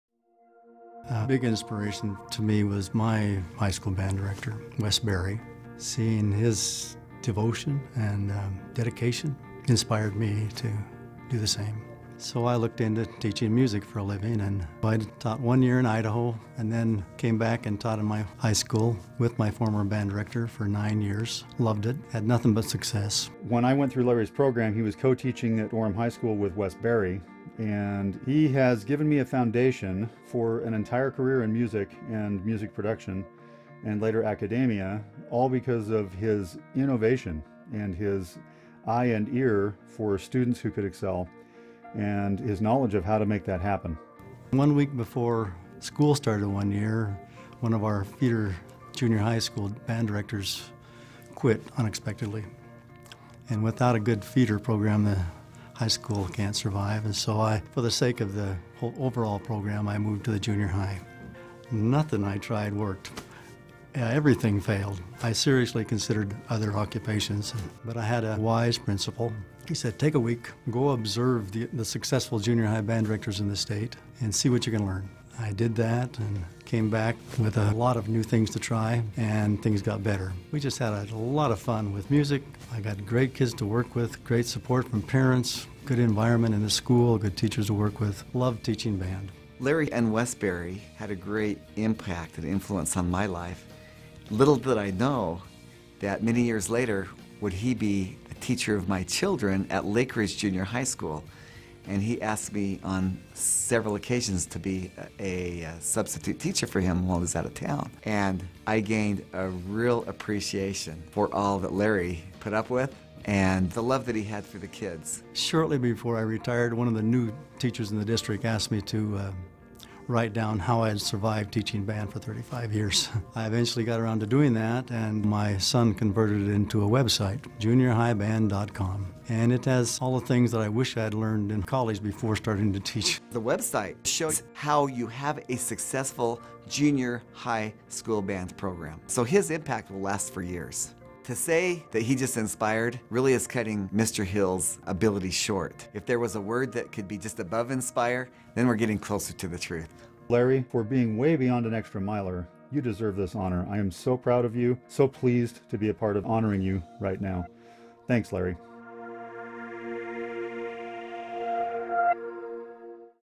Mr. Hill - Red Cross Award Video
A video tribute to Mr. Larry Hill presented at the American Red Cross Hero Recognition Event for Utah in 2015.  Mr. Hill was the recipient of the Educator Hero award.

for more information about Mr. Hill and his proven techniques for creating a successful junior high/middle school band program.